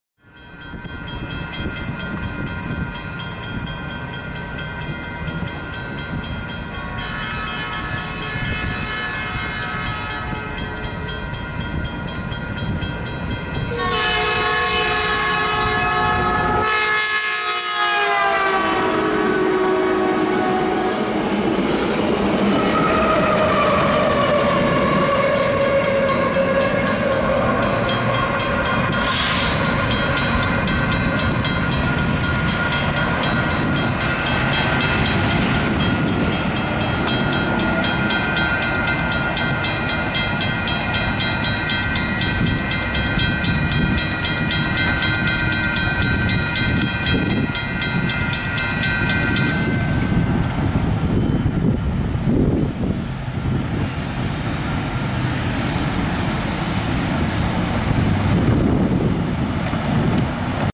*RARE* Metrolink 864 (with BNSF 5615) @ Rose Ave (Oxnard, CA)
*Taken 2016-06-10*

This is one of the few Metrolinks I got on film with the BNSF leasers they had for a while.